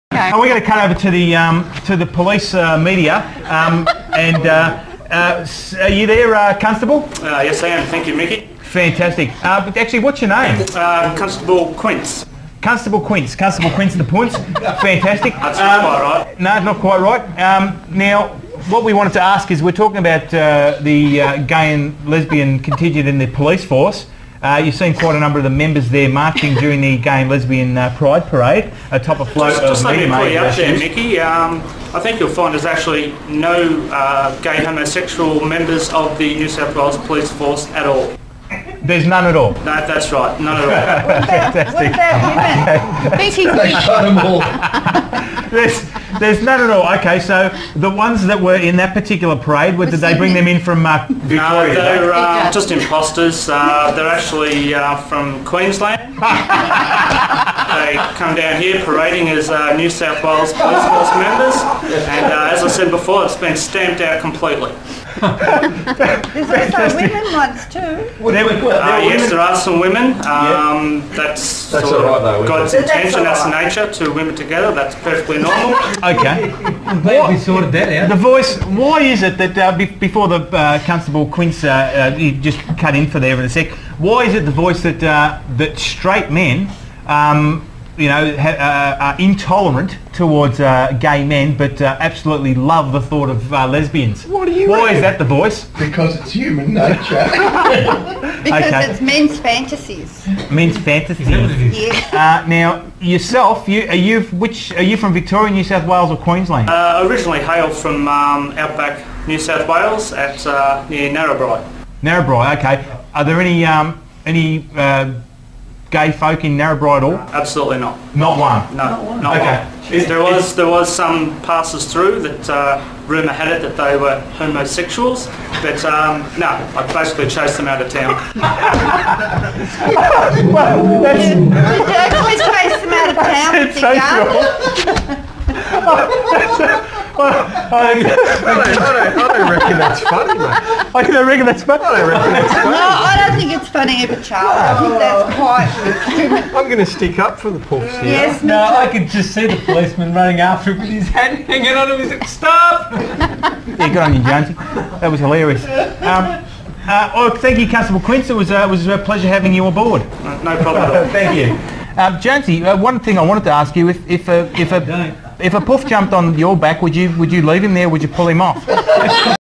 Gay Police Officers in New South Wales
There's no official statistics revealing the number of gay police in the NSW Police Force. Police Gay and Lesbian Liaison Officers (GLLOs) participate in a range of events including Fair Day in Victoria Park and the Mardi Gras Parade in Darlinghurst.